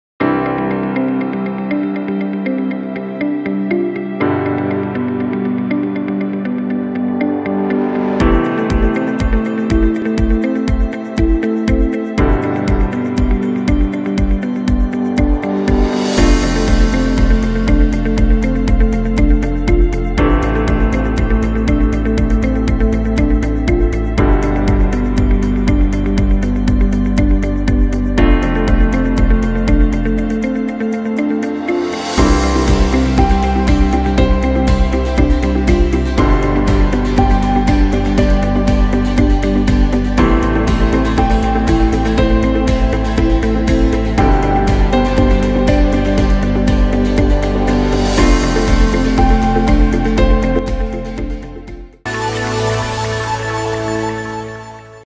What Makes a Person? Book Trailer
A highly readable insight into the secret events of our first 1,000 days of life. Events that are unique to each of us and affect the control of our bodies, the diseases we experience, and the chance of a healthy future for our children. A surprising book with implications for young people, parents and societies.